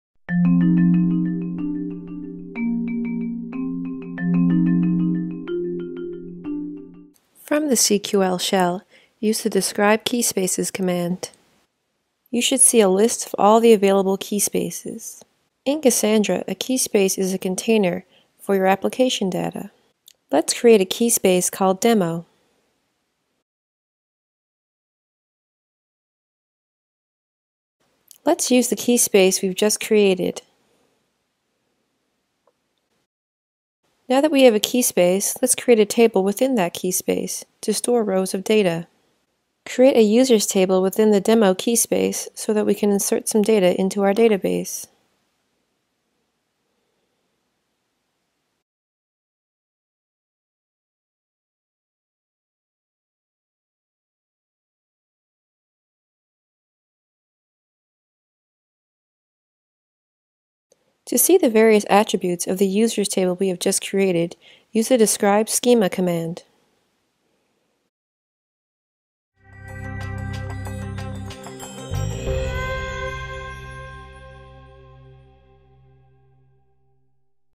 Cassandra Developer Walkthrough — Step 1: Create a Keyspace and Table
What You'll Learn: Gain and understanding of what a 'keyspace' is in Cassandra and its similarities to a database schema, as well as creating tables in your Cassandra database.